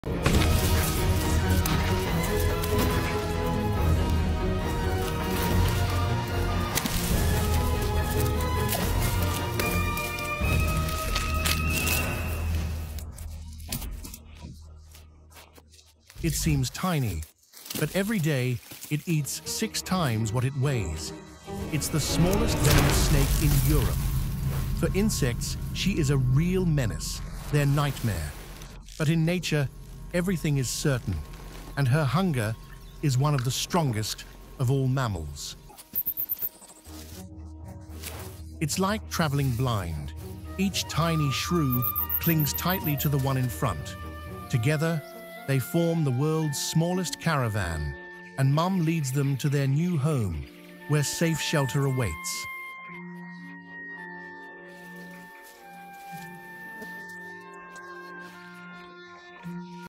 How the smallest predators on the planet live
In this video we will talk about the smallest venomous snake in Europe, whose need for food is amazing! And we will also show a fascinating scene of a shrew's journey as the babies follow their mother, forming an unusual caravan. Learn more about the life and survival of these tiny but strong creatures.